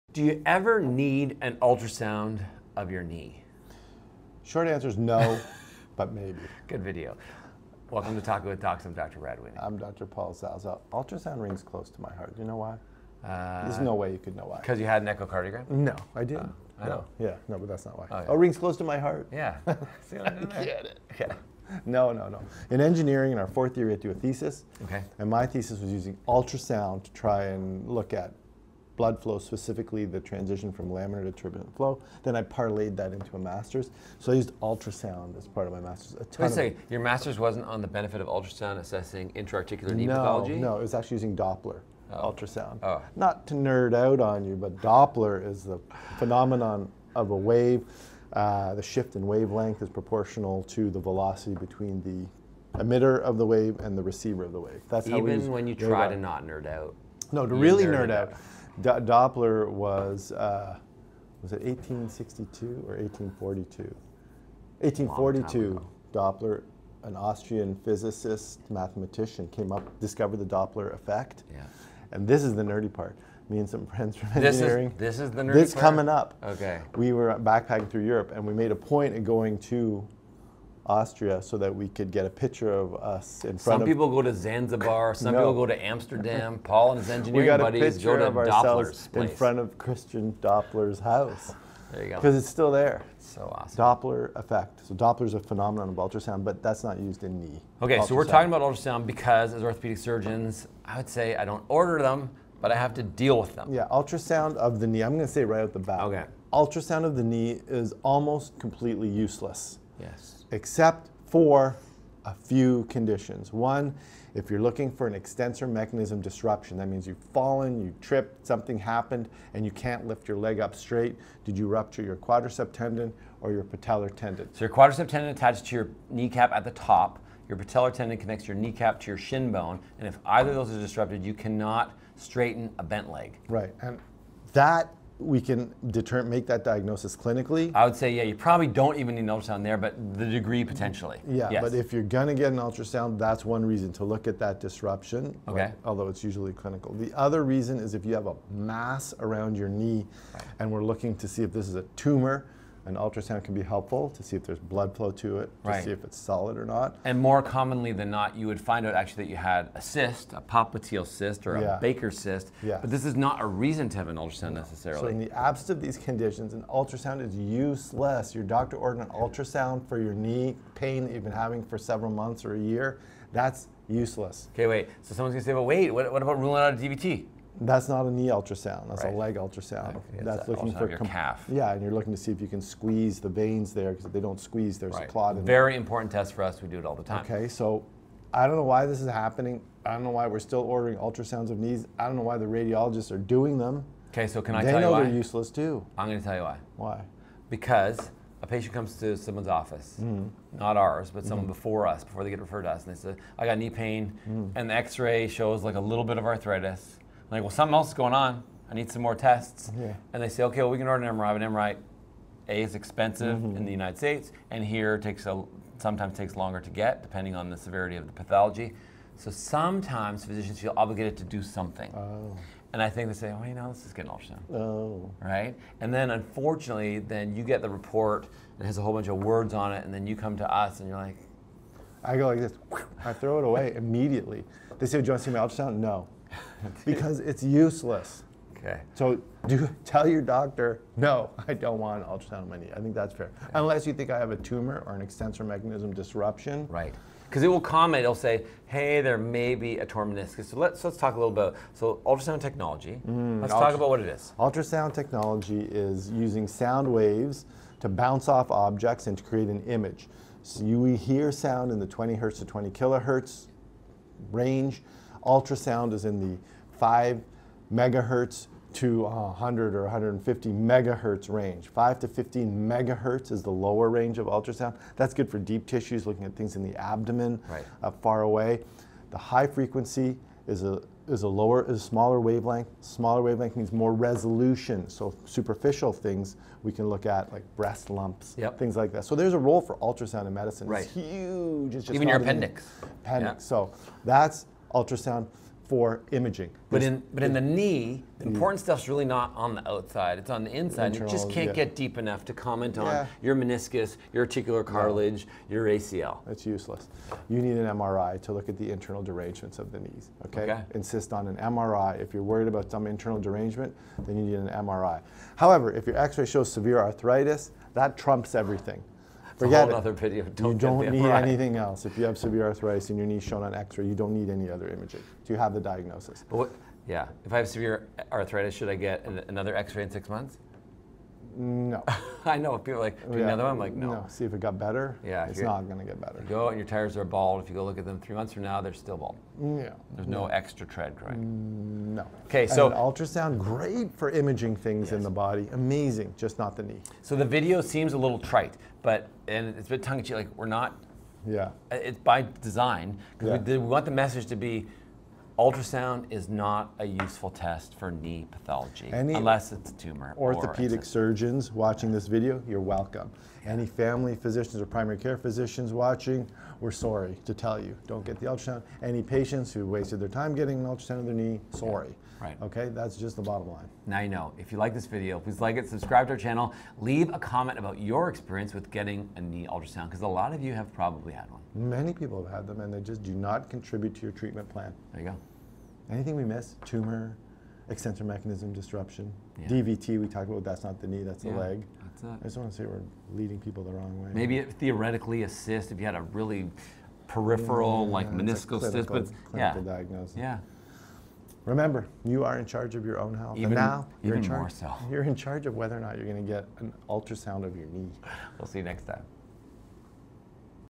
Is There Any Reason To Get A Knee Ultrasound?
In this episode, doctors break down the truth about knee ultrasounds — and why they’re often not the best tool for diagnosing knee pain. From sports injuries to arthritis, many patients are surprised when their ultrasound results don’t offer any clear answers.

So… are knee ultrasounds actually useless? Not quite — but they’re also not as helpful as you might think.

We explain how ultrasound technology works, why doctors rarely rely on it for knee issues, and when it actually is the right imaging choice. Plus, we discuss what scans are better for spotting meniscus tears, ligament injuries, and more.

You’ll learn:

- Why ultrasounds are limited when it comes to joints like the knee

- What an ultrasound can and can’t show

- When knee ultrasounds are medically appropriate

- The differences between ultrasound, MRI, and X-ray

- Why some patients are prescribed ultrasounds anyway

- How ultrasound technology actually works to create images

If you’ve ever been told to get an ultrasound for knee pain — or you’re wondering what scan is best — this episode will help you understand what’s truly useful (and what’s not) in modern knee diagnostics.

YouTube Music: • TWD Podcast

References:
Coming soon — full list of imaging studies and clinical guidelines on musculoskeletal ultrasound use.